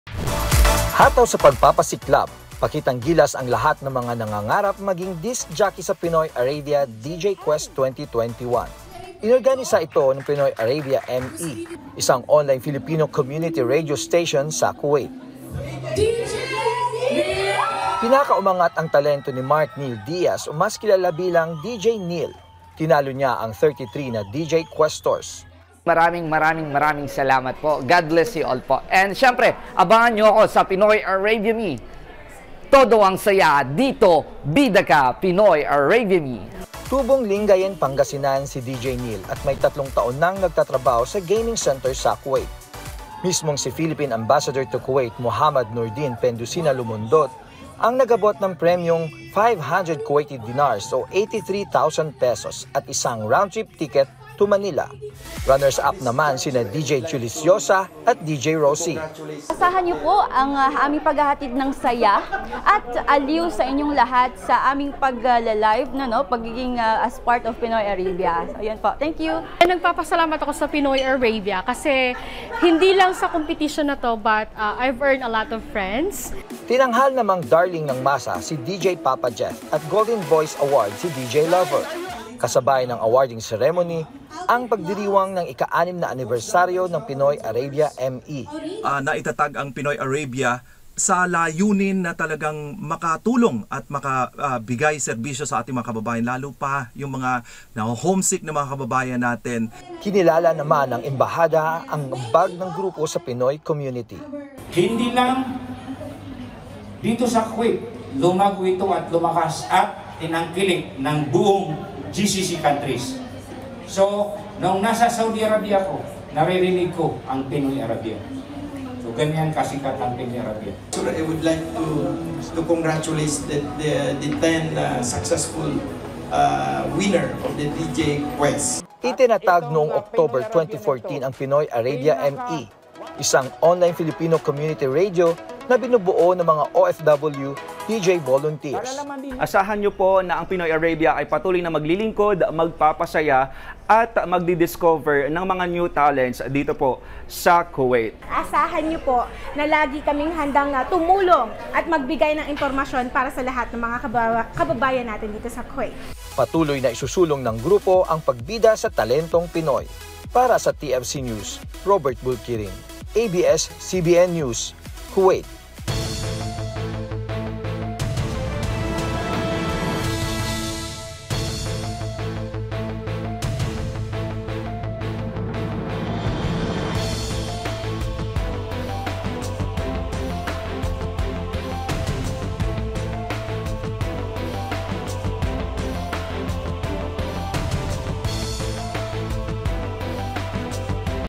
Mga Pinoy sa Kuwait nagtagisan sa DJ Quest Competition | TFC News Kuwait
Nagtagisan ng galing ang ilang Pinoy sa DJ Quest Competition sa Kuwait. Inorganisa ito ng isang online Filipino community radio station sa Kuwait. Nagpapatrol, Robert Bulquiren.